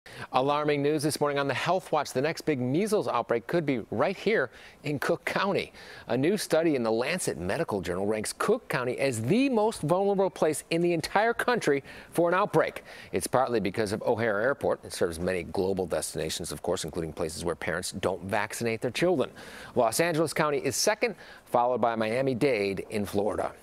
New Medical Study Names Cook County Most Vulnerable To Measles Outbreak
The ranking is partly because of O'Hare International Airport, which serves many destinations, including places where parents don't vaccinate their children.